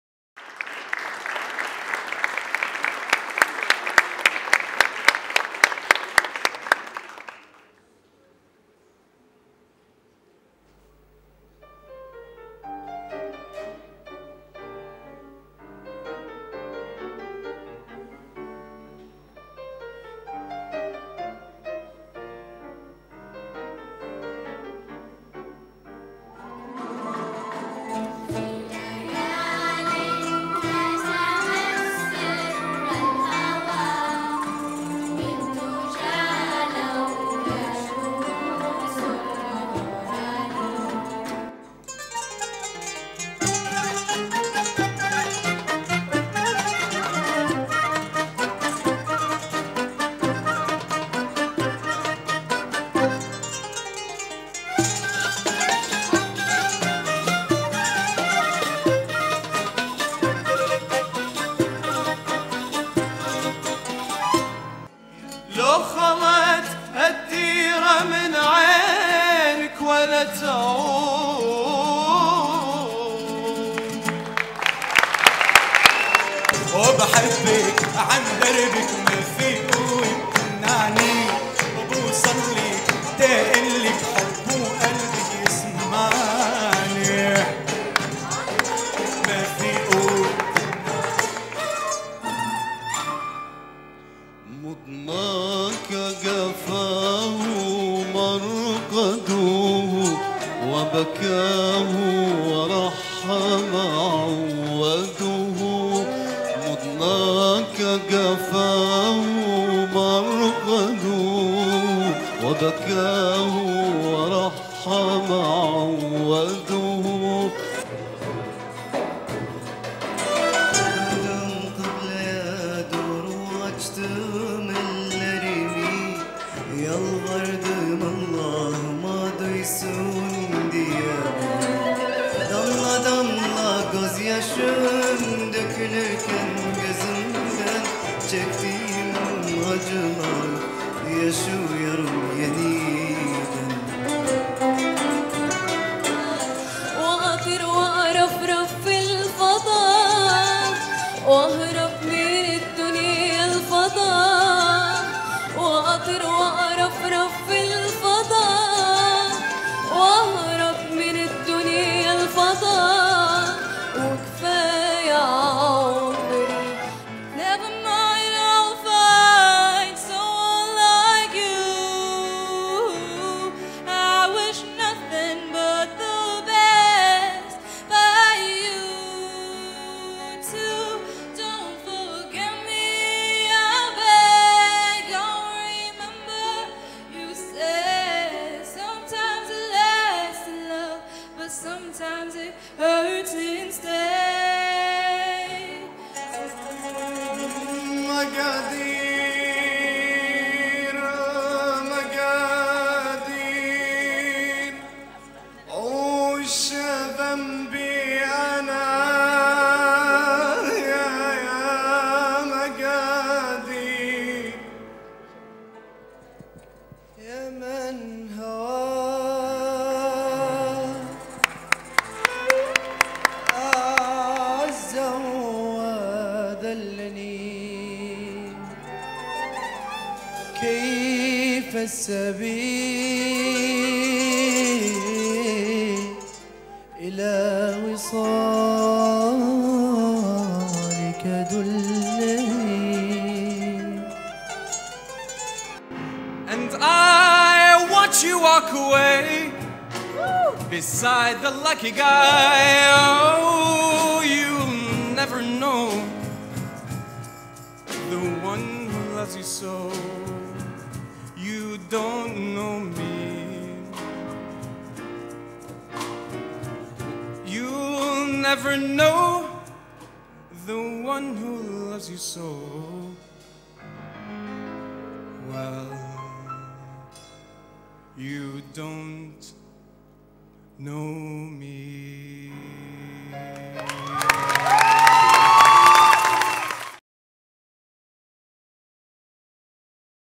Aswat Shabah
The LOYAC Academy of Performing Arts performed at Dar Al Athaar Al Islamiyah with their end of year show Aswat Shabah. Here are some highlights, Enjoy!